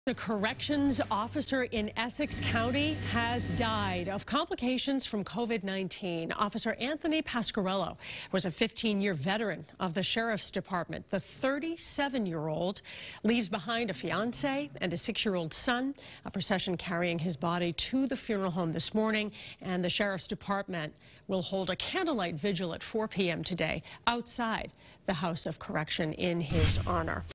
Essex County Sheriff's Deputy Dies Of Complications From COVID-19
Correctional Officer Anthony J. Pasquarello of Saugus has died at the age of 37 due to complications from COVID.